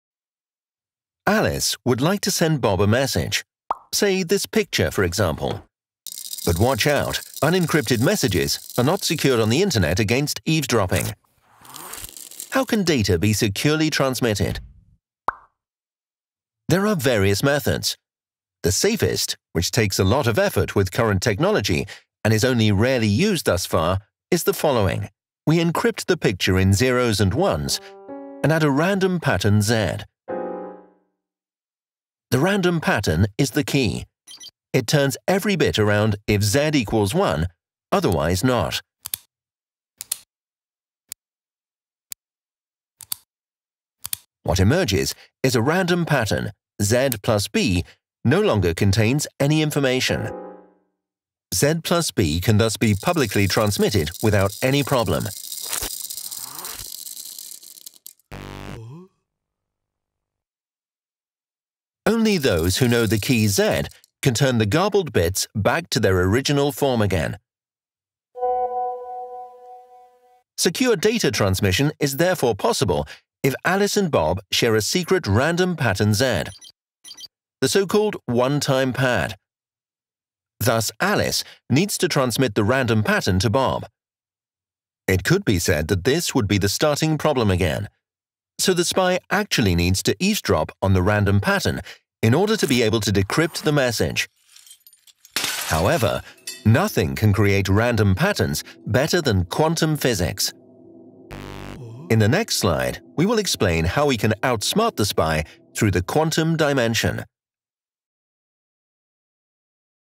One time pads for quantum cryptography (U3-02-02)
How to hide informations in randomness.

All videos of the project Quantum Visions can be found